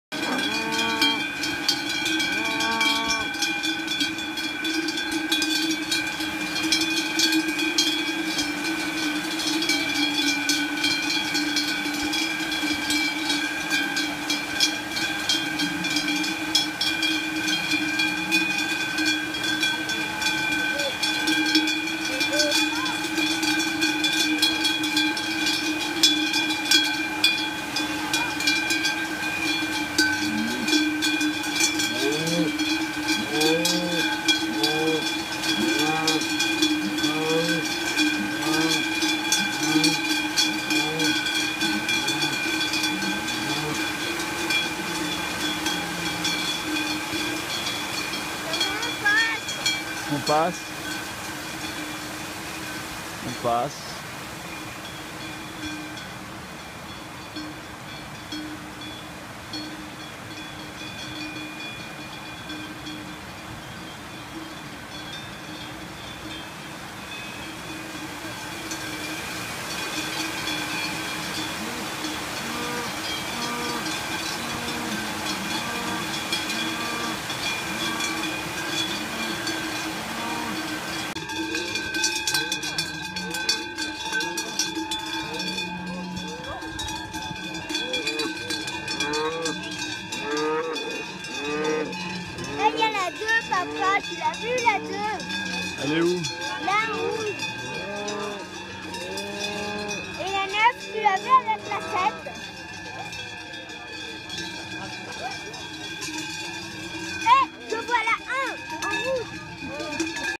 Inalpe à Novèlett | 2015 | Hérens | V4S | Valais | Suisse
Sur les hauts de Cotter, au-dessus de Villaz (Evolène) les vaches de la race d'Hérens déterminent en combattant laquelle sera la dominante du troupeau à l'alpage.

Our only Intent is that you enjoy viewing this video.